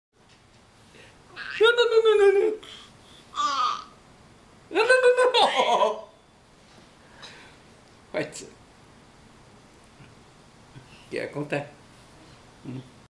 Aviva steals Agong's glasses
Aviva's having a great time with Agong, especially after she manages to snatch his glasses